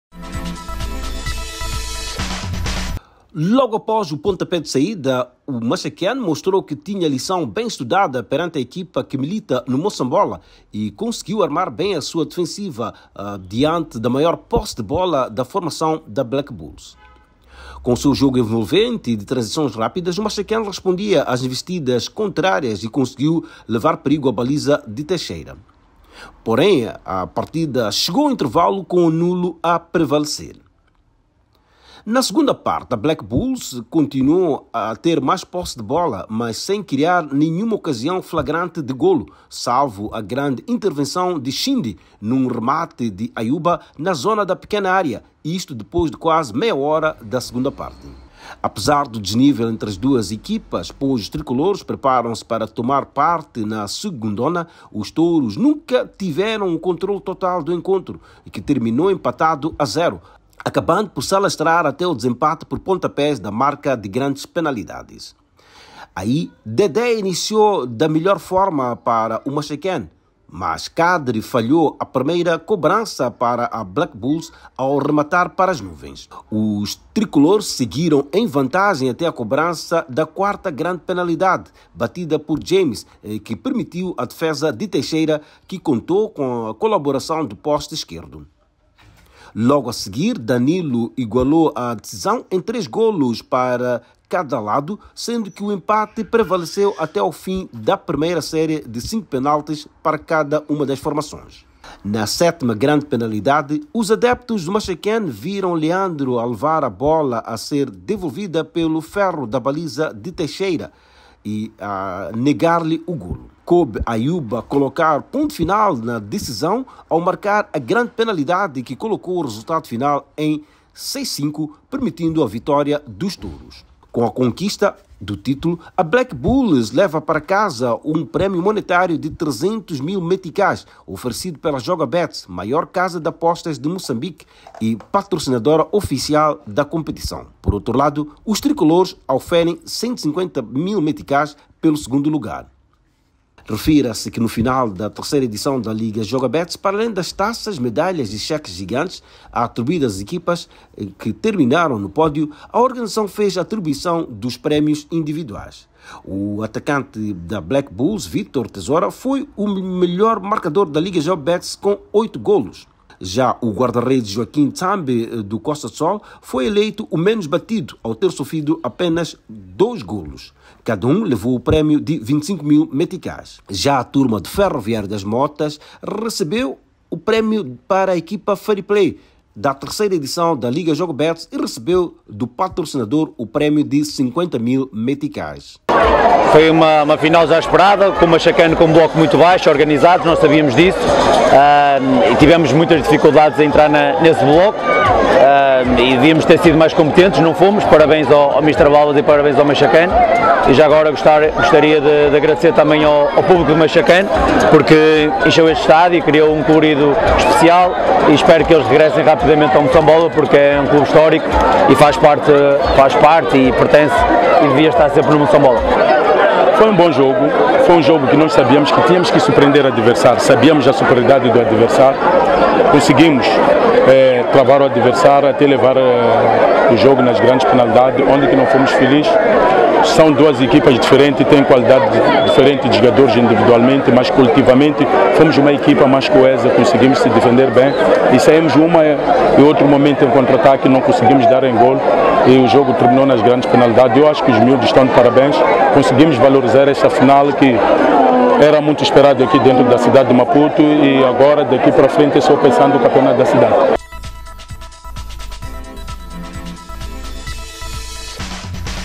FINAL LIGA JOGABETS: BLACK BULLS 0 (6-5) 0 MAXAQUENE
FINAL LIGA JOGABETS: BLACK BULLS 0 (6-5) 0 MAXAQUENE : A Associação Black Bulls suou e precisou recorrer ao desempate por grandes penalidades (6-5) para derrotar o Maxaquene e conquistou o título de campeão da Liga Jogabets, prova também designada de Campeonato Provincial da Cidade de Maputo que marca a abertura da época na capital do país, num encontro que teve lugar na tarde deste sábado (06) no Campo do Sol. É pela segunda vez consecutiva que os “touros” conquistam a Liga Jogabets.